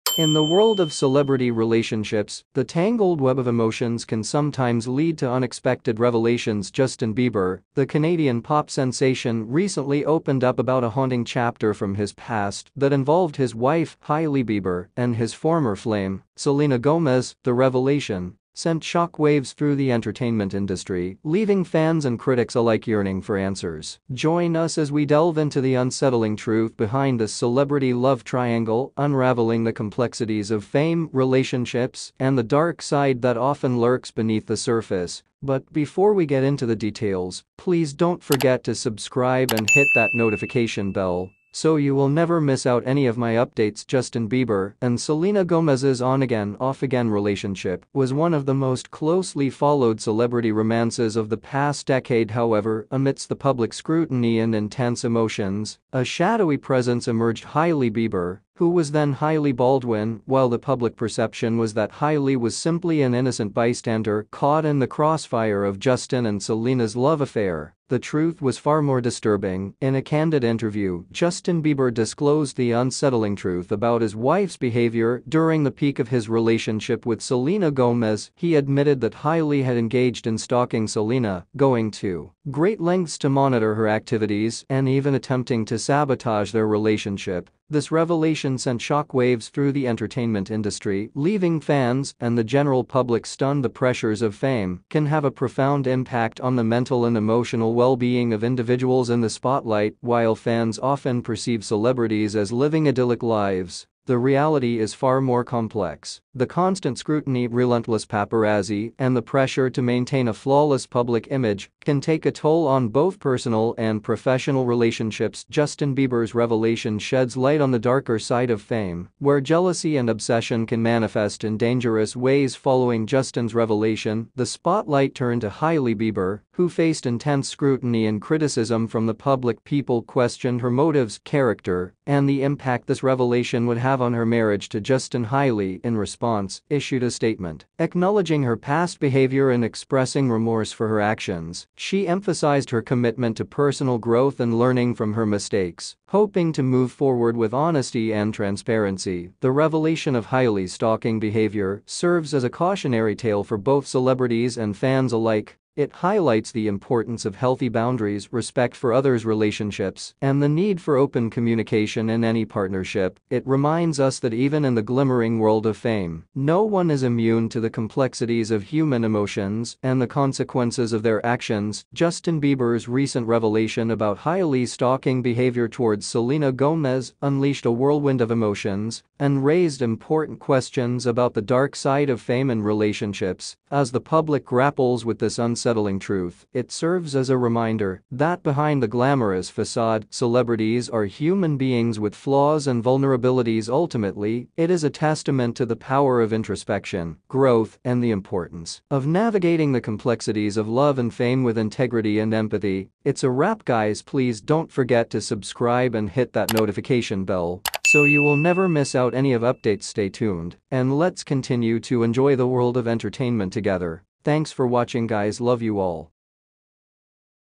Justin Bieber Reveals The Scary Truth Behind Hailey Stalking Selena Gomez
The content is based on celebrity vibes and lifestyle.Hope you really enjoy them, you can check my channel for more updates and celebrity news.